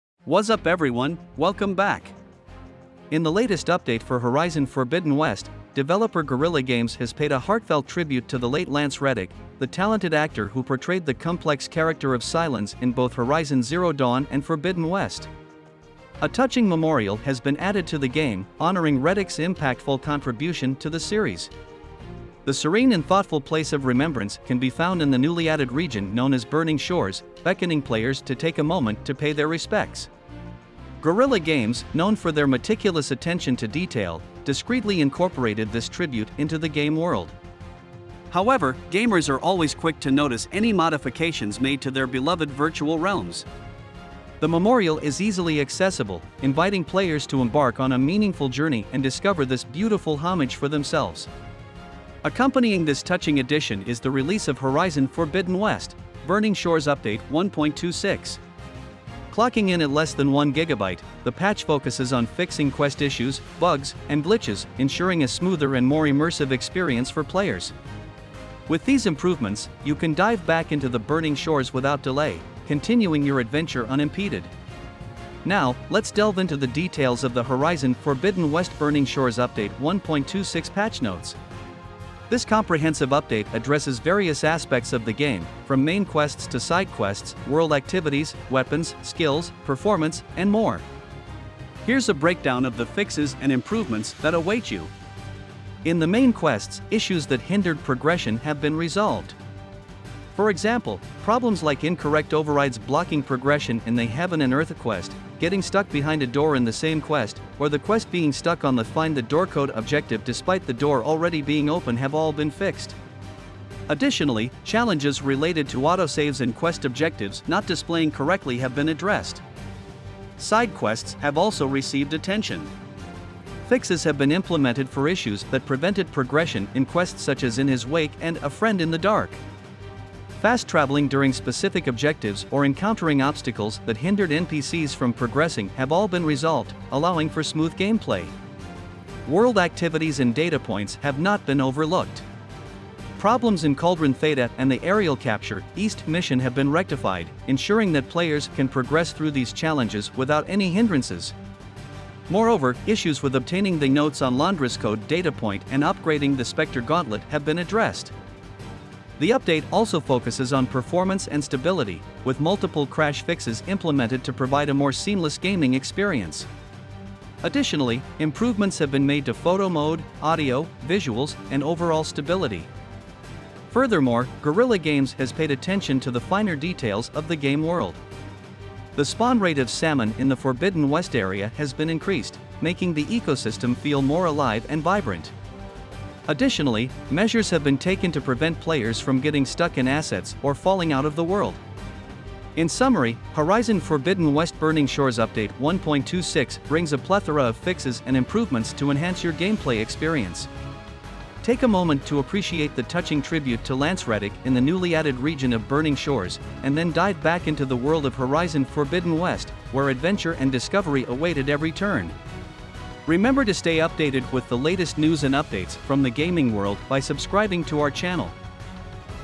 Horizon Forbidden West Update 1.26 Adds Beautiful Tribute to Lance Reddick
Wazzup Everyone, Welcome Back!

In the latest update for Horizon Forbidden West, developer Guerilla Games has paid a heartfelt tribute to the late Lance Reddick, the talented actor who portrayed the complex character of Sylens in both Horizon Zero Dawn and Forbidden West. A touching memorial has been added to the game, honoring Reddick's impactful contribution to the series. The serene and thoughtful place of remembrance can be found in the newly-added region known as Burning Shores, beckoning players to take a moment to pay their respects.

Guerilla Games, known for their meticulous attention to detail, discreetly incorporated this tribute into the game world. However, gamers are always quick to notice any modifications made to their beloved virtual realms. The memorial is easily accessible, inviting players to embark on a meaningful journey and discover this beautiful homage for themselves.

Accompanying this touching addition is the release of Horizon Forbidden West: Burning Shores update 1.26. Clocking in at less than 1GB, the patch focuses on fixing quest issues, bugs, and glitches, ensuring a smoother and more immersive experience for players. With these improvements, you can dive back into the Burning Shores without delay, continuing your adventure unimpeded.

Now, let's delve into the details of the Horizon Forbidden West Burning Shores update 1.26 patch notes. This comprehensive update addresses various aspects of the game, from main quests to side quests, world activities, weapons, skills, performance, and more. Here's a breakdown of the fixes and improvements that await you:

In the main quests, issues that hindered progression have been resolved. For example, problems like incorrect overrides blocking progression in the "Heaven and Earth" quest, getting stuck behind a door in the same quest, or the quest being stuck on the "Find the Door Code" objective despite the door already being open have all been fixed. Additionally, challenges related to autosaves and quest objectives not displaying correctly have been addressed.

Side quests have also received attention. Fixes have been implemented for issues that prevented progression in quests such as "In His Wake" and "A Friend in the Dark." Fast traveling during specific objectives or encountering obstacles that hindered NPCs from progressing have all been resolved, allowing for smooth gameplay.

World activities and datapoints have not been overlooked. Problems in Cauldron "THETA" and the "Aerial Capture: East" mission have been rectified, ensuring that players can progress through these challenges without any hindrances. Moreover, issues with obtaining the "Notes on Londra's Code" datapoint and upgrading the Specter Gauntlet have been addressed.

The update also focuses on performance and stability, with multiple crash fixes implemented to provide a more seamless gaming experience. Additionally, improvements have been made to photo mode, audio, visuals, and overall stability.

Furthermore, Guerilla Games has paid attention to the finer details of the game world. The spawn rate of salmon in the Forbidden West area has been increased, making the ecosystem feel more alive and vibrant. Additionally, measures have been taken to prevent players from getting stuck in assets or falling out of the world.

In summary, Horizon Forbidden West Burning Shores update 1.26 brings a plethora of fixes and improvements to enhance your gameplay experience. Take a moment to appreciate the touching tribute to Lance Reddick in the newly-added region of Burning Shores, and then dive back into the world of Horizon Forbidden West, where adventure and discovery await at every turn.